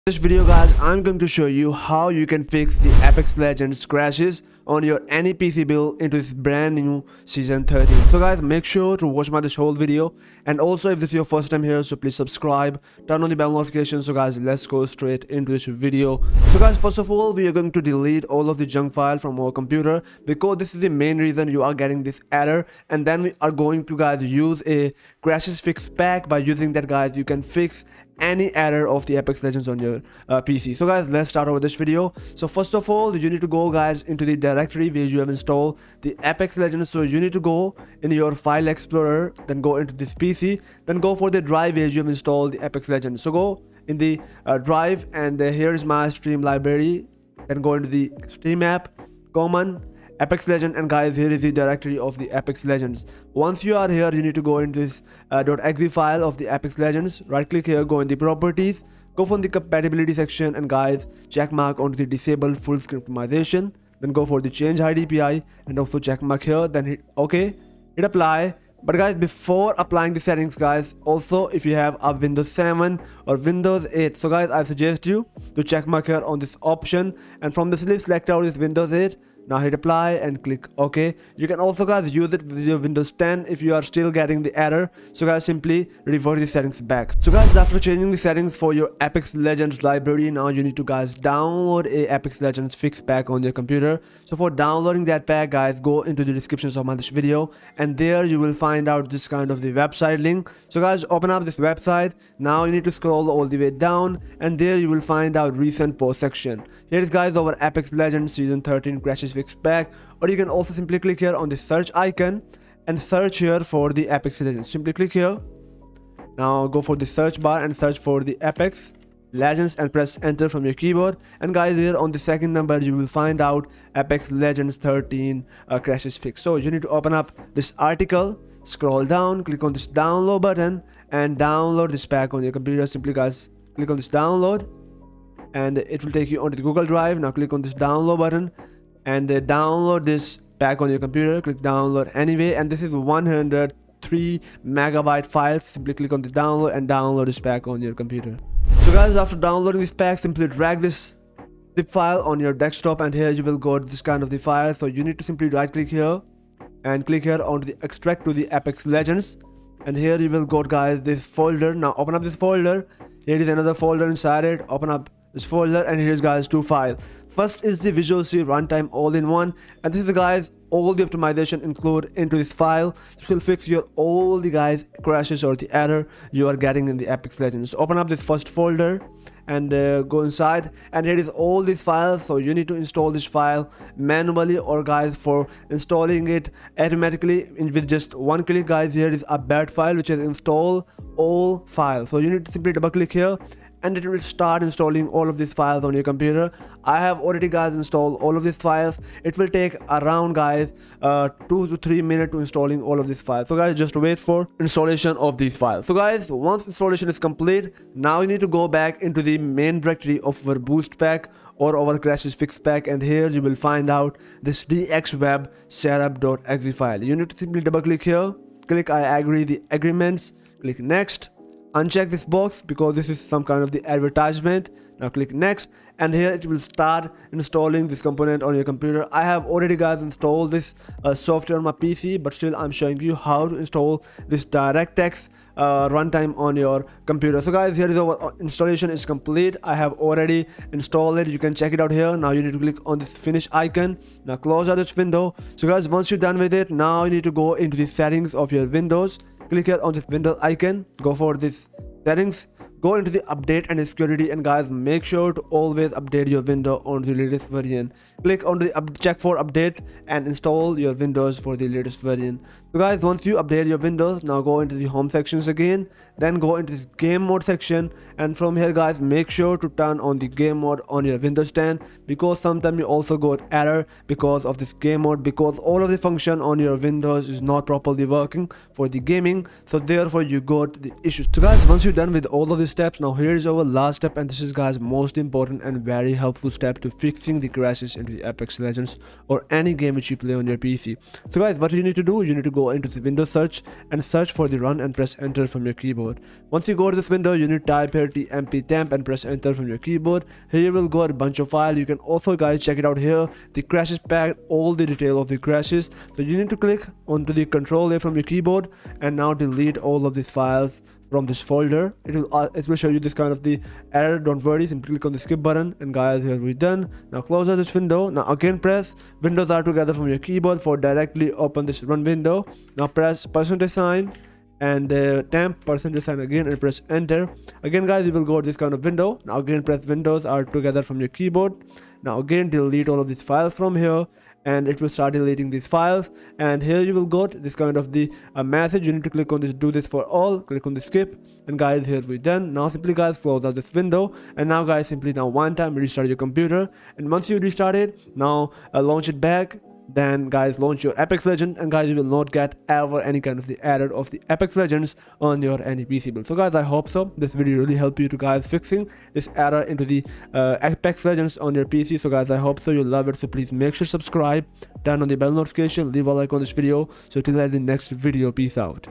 Apex Legends Season 13: How to Fix Engine Error, DirectX Error & DXGI_ERROR_DEVICE_HUNG in PC
Hello People!
Today in this video, I am showing you Apex Legends Season 13: How to Fix Engine Error, DirectX Error & DXGI_ERROR_DEVICE_HUNG on PC

Title: Apex Legends Season 13: How to Fix Engine Error, DirectX Error & DXGI_ERROR_DEVICE_HUNG in PC

INTRO 0:00
Game Directory Settings 00:17
Crash Fix Pack (Important) 1:27
Windows Settings 4:05
Delete Junk Files 4:40
End 6:09